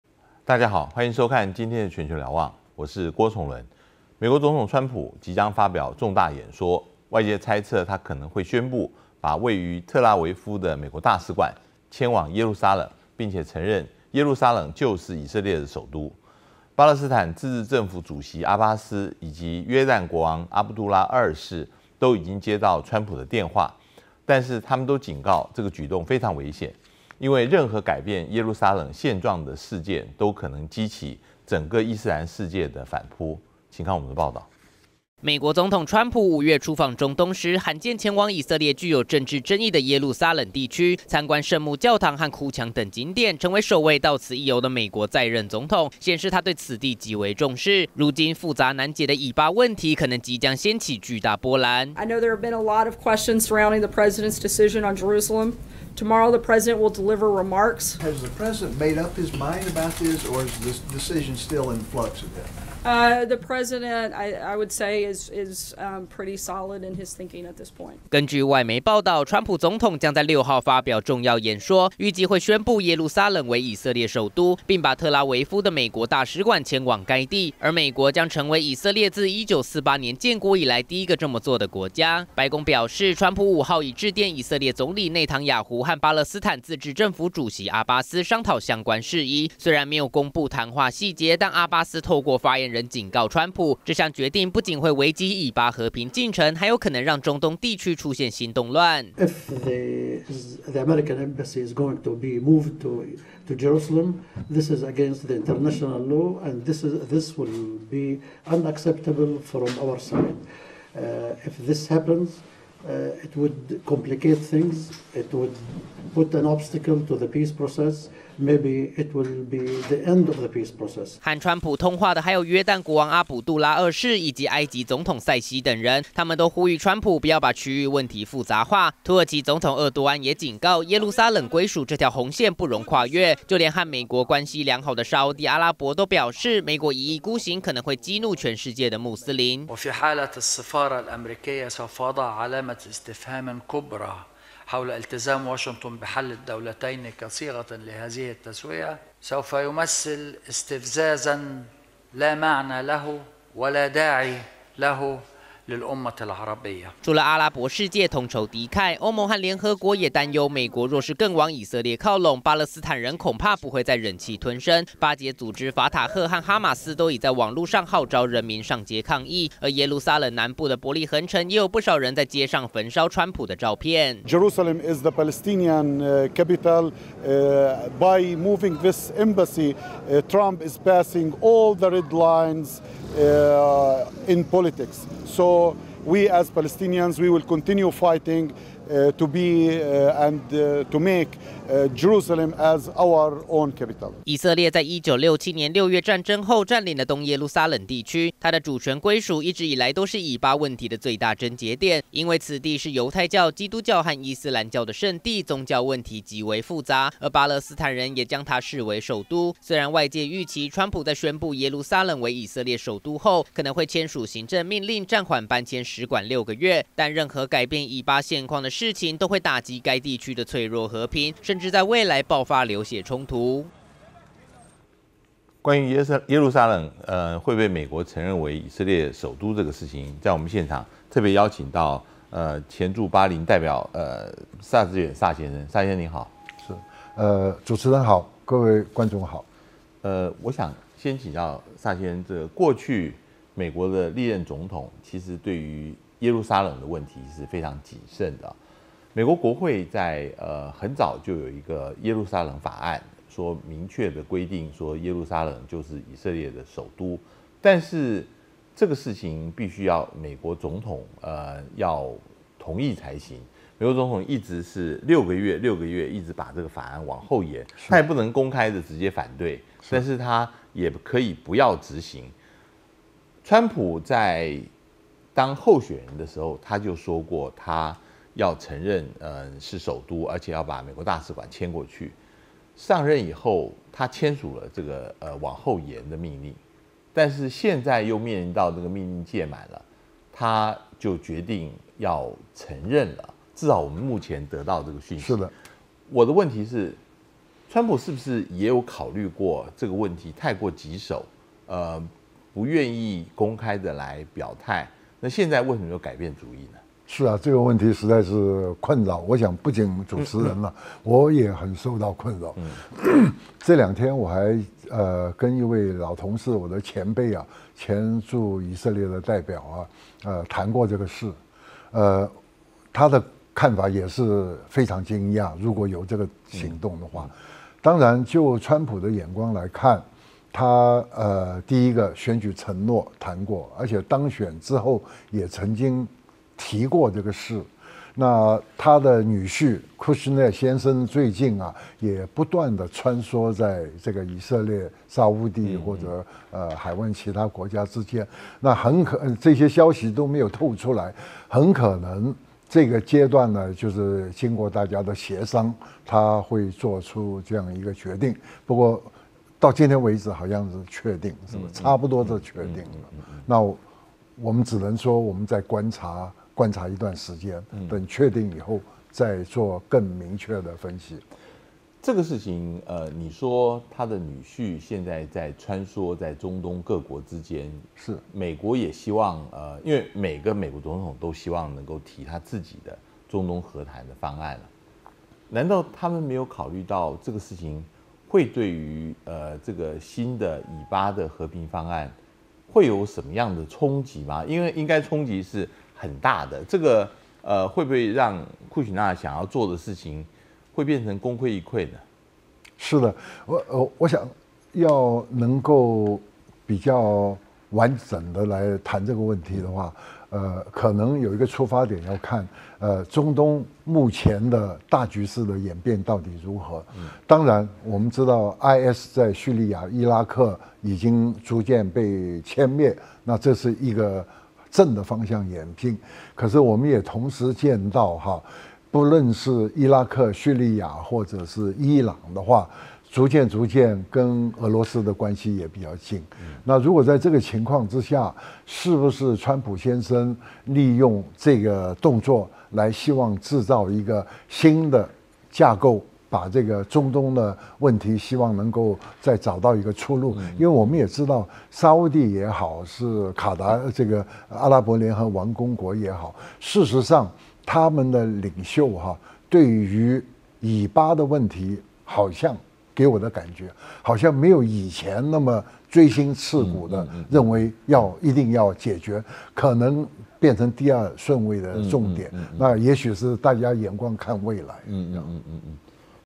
1206 認耶路撒冷是首都？川普挺以恐引戰中東
美國總統川普是否將美國駐以色列大使館從特拉維夫遷至耶路撒冷備受矚目，川普在競選期間，表示要承認耶路撒冷為首都，並要遷移大使館過去，後來就任後，為什麼仿效過去20多年來，歷屆政府都會定期簽署，延緩搬遷的行動？現在延緩命令到期，是否代表他不願意再延？